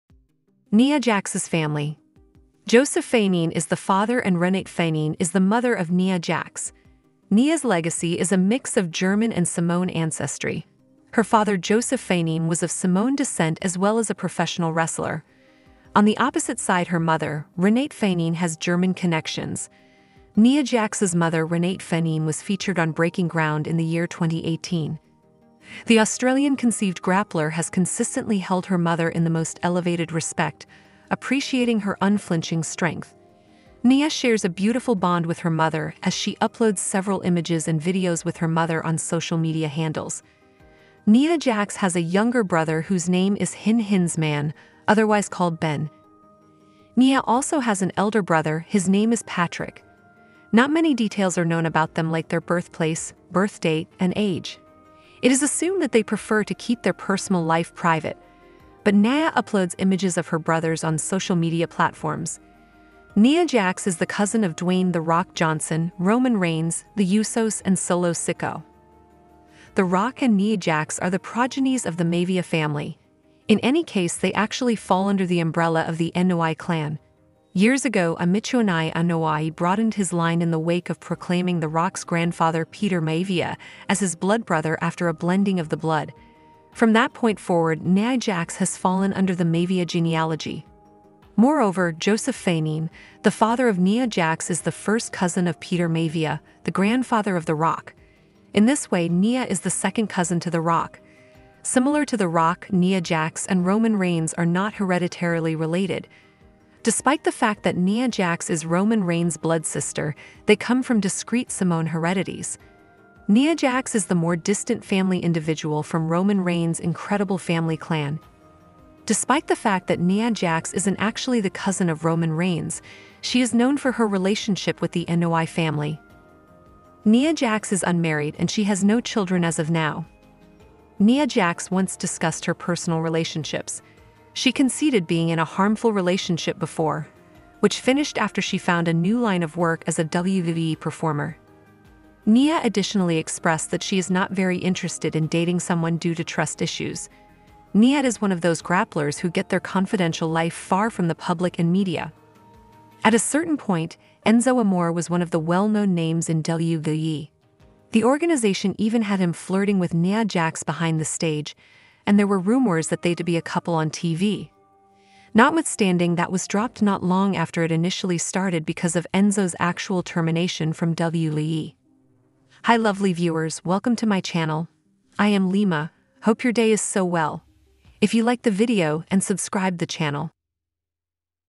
Big update! Who are Nia Jax's family! legacy is a mix of German and Samoan ancestry! VERY sad
⚪WW FAMILY

⚪WW FAMILY